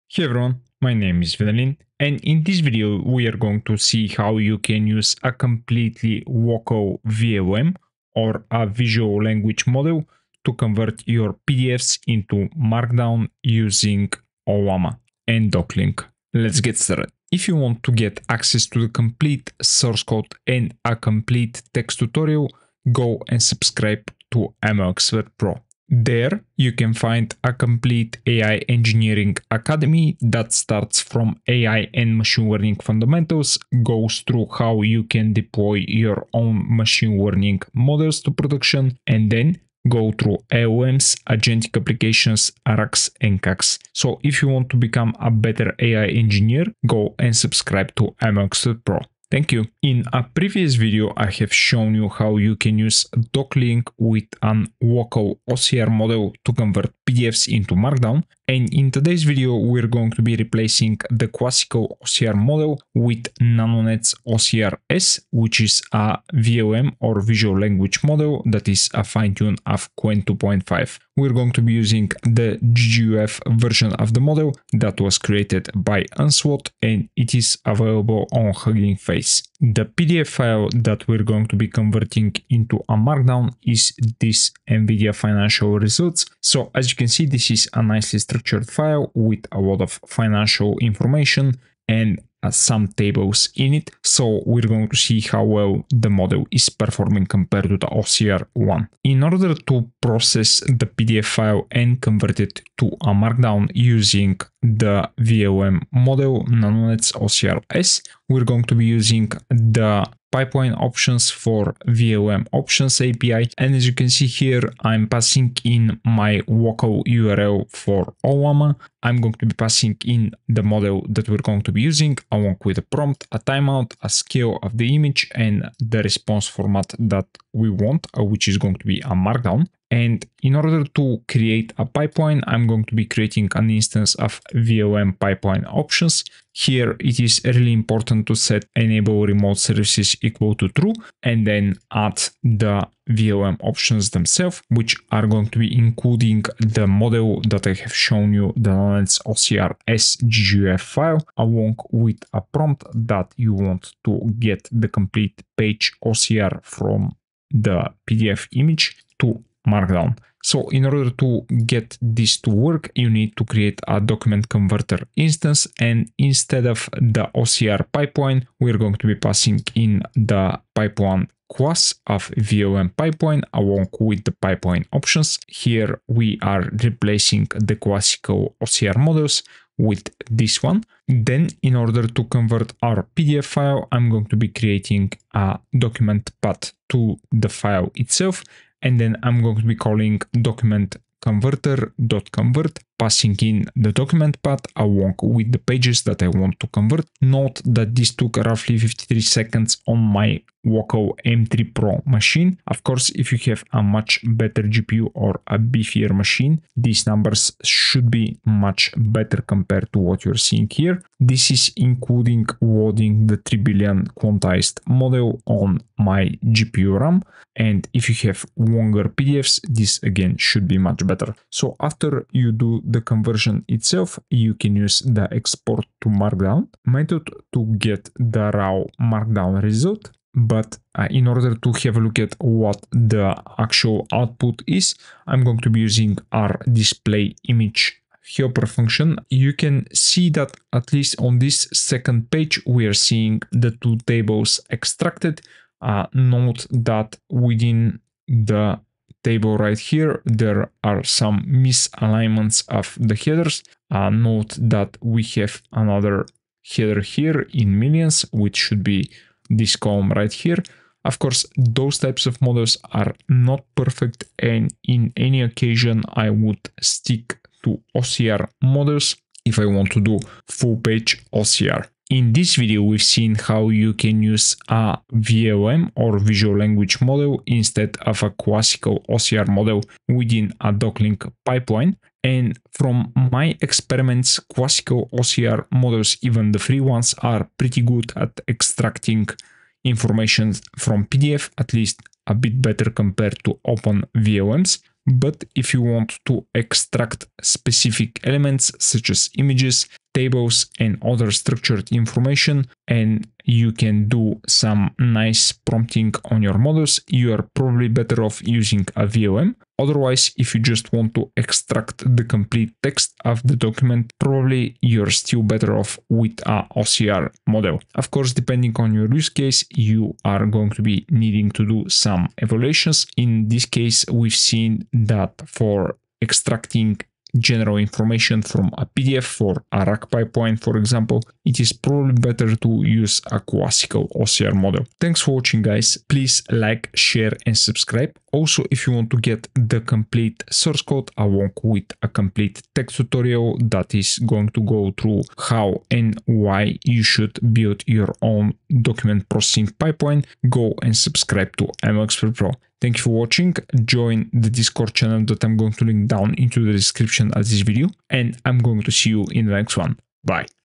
100% Local PDF OCR with Docling and Ollama | PDF to Markdown with VLM (Nanonets-OCR-s)
How to privately and locally convert PDFs into nicely structured Markdown files? Docling allows you to do it. On top of that, you can use a local Visual Language Model (with Ollama) for OCR extraction instead of classical OCR models. Are we going to get better results?